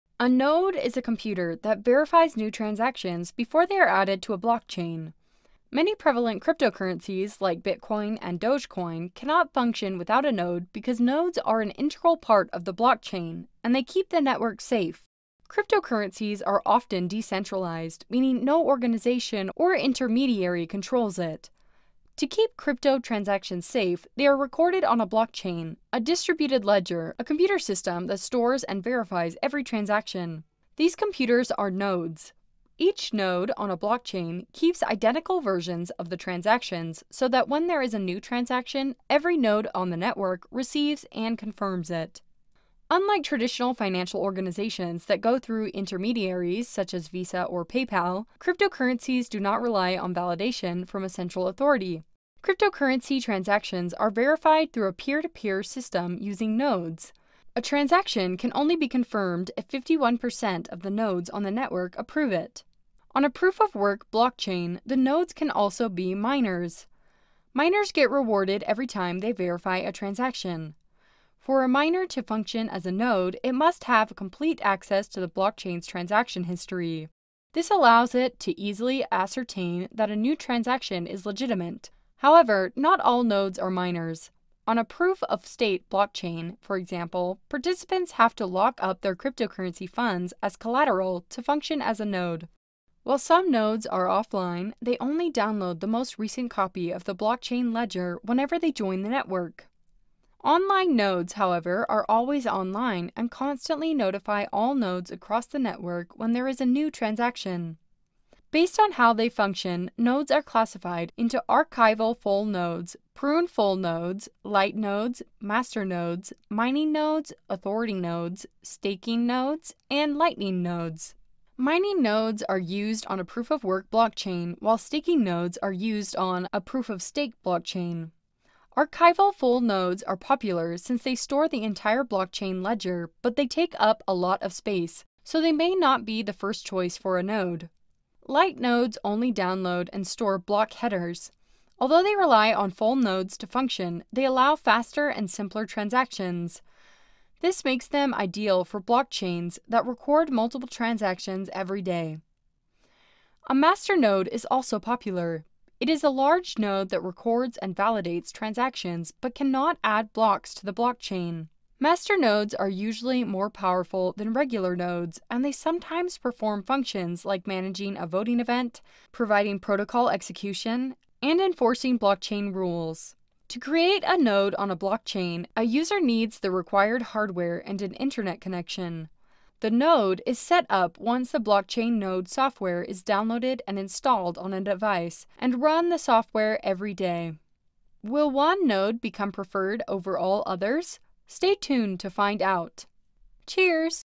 What is a Node? EVERYTHING YOU NEED TO KNOW
What is a Node?

In this video, we will be diving into the world of cryptocurrency and exploring the concept of a node. A node is a crucial component of a blockchain network, serving as a connection point for users to communicate with the network and validate transactions.

Nodes play a vital role in ensuring the security and decentralization of a blockchain network. They help to distribute the network's data, validate transactions, and maintain the network's integrity.

There are different types of nodes, including full nodes, light nodes, and super nodes. Full nodes store a complete copy of the blockchain and validate transactions, while light nodes only store a subset of the data and rely on other nodes for validation. Super nodes have a higher level of power and influence in the network, and are often used for tasks such as block validation and consensus-building.

To become a node, an individual must run a node software on their computer and participate in the network by contributing their computing power and storage capacity. By doing so, nodes help to support the network, increase its security, and improve its overall performance.

So, whether you are a seasoned crypto enthusiast or just starting to learn about the world of blockchain and cryptocurrency, understanding what a node is and its role in a network is essential.

Curiously Crypto is for general information purposes and is not intended to be and should not be taken as legal advice or financial advice.